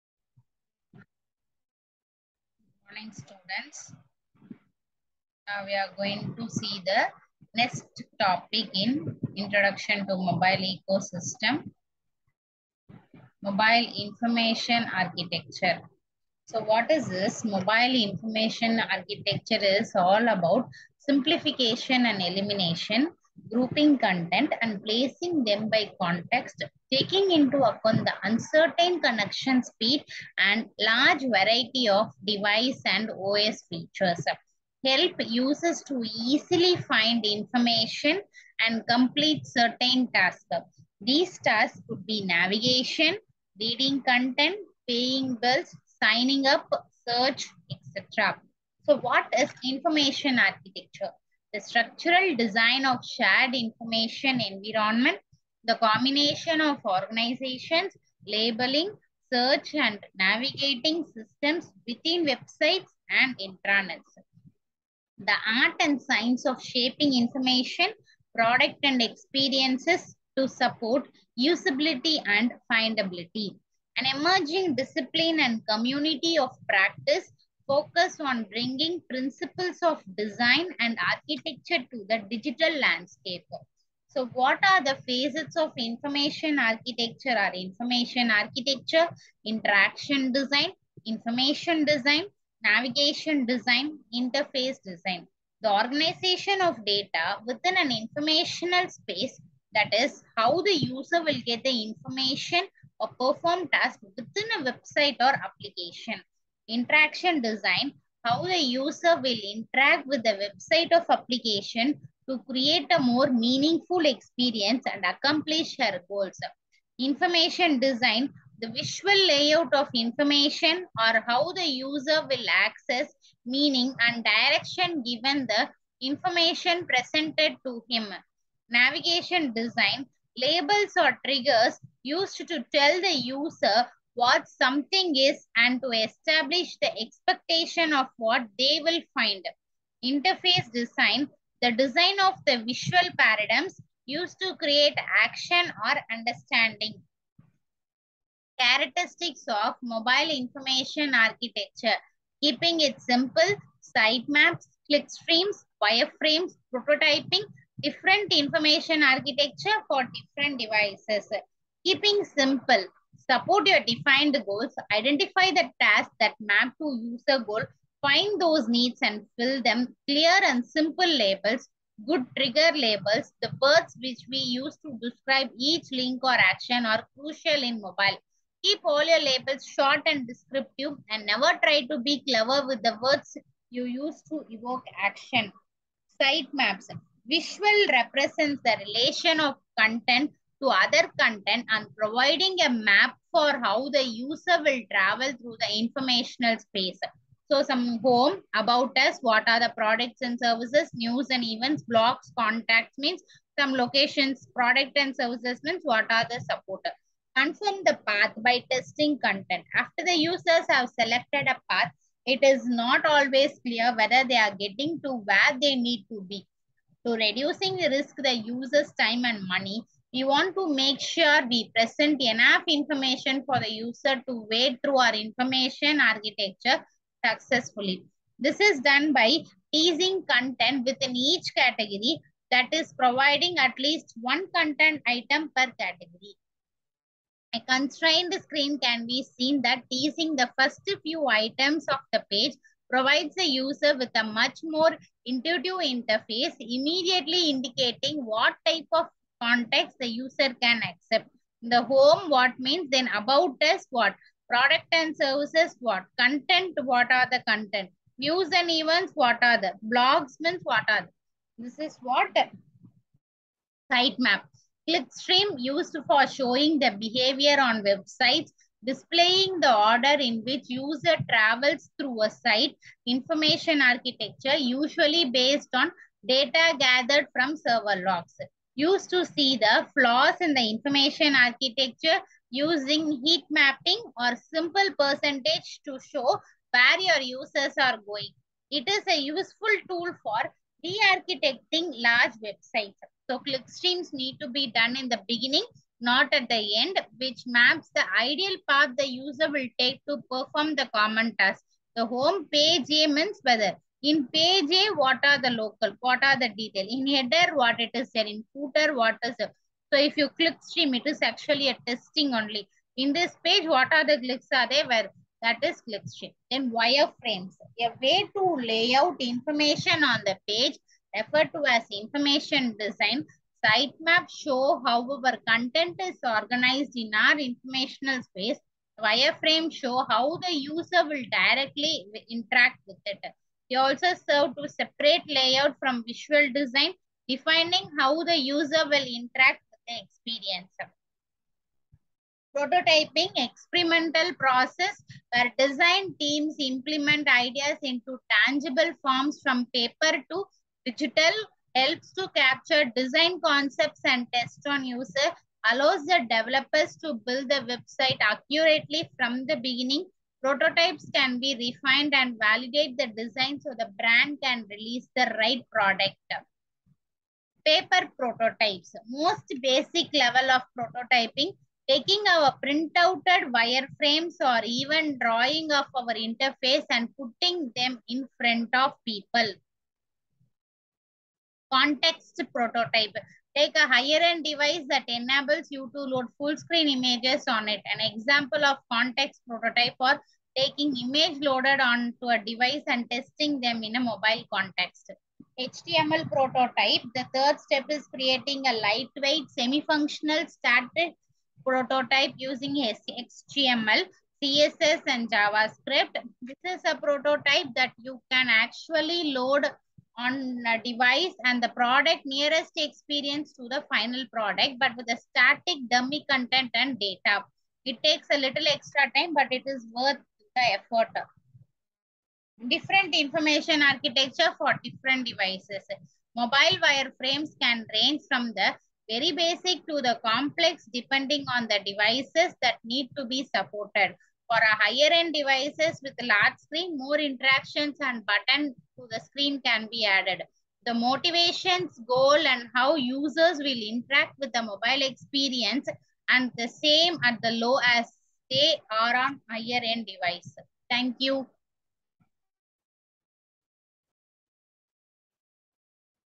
Information architecture - Human Computer Interaction , Mrs. G. Manisha, AP/CSE, RMDEC
This video explains about the Information architecture in Human Computer Interaction